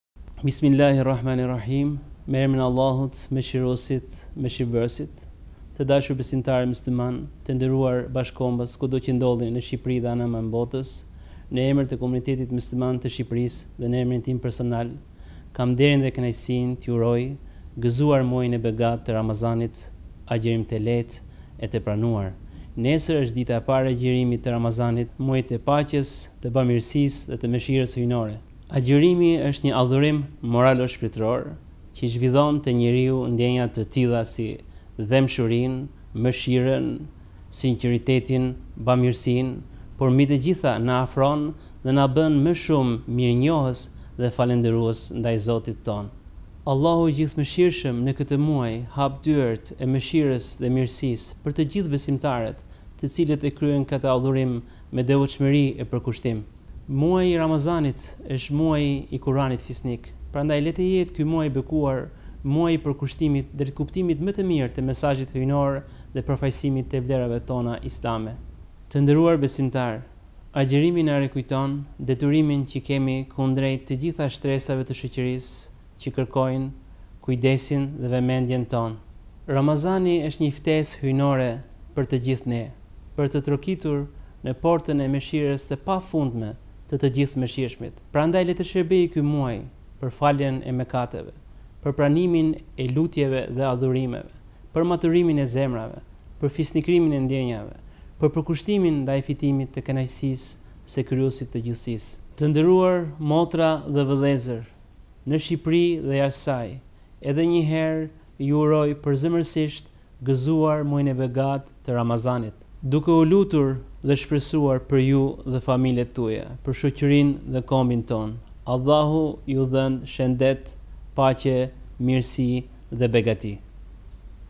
Urimi i kryetarit të KMSH-së z. Skender Bruçaj për Muajin e Ramazanit
Në emër të Komunitetit Mysliman të Shqipërisë dhe në emrin tim personal, kam kënaqësinë dhe nderin t’Ju uroj:
Gëzuar muajin e begatë të Ramazanit. Agjërim të lehtë e të pranuar.
Nesër është dita e parë e agjërimit të Ramazanit, muajit të paqes, solidaritetit dhe Mëshirës Hyjnore.
Agjërimi është një institucion moralo-shpirtëror, që zhvillon te njeriu ndjenja të tilla si: mëshirën, dhembshurinë, respektin ndaj tjetrit, bamirësinë dhe sinqeritetin, por, mbi të gjitha, na afron dhe na bën më shumë falënderues e mirënjohës ndaj Zotit tonë.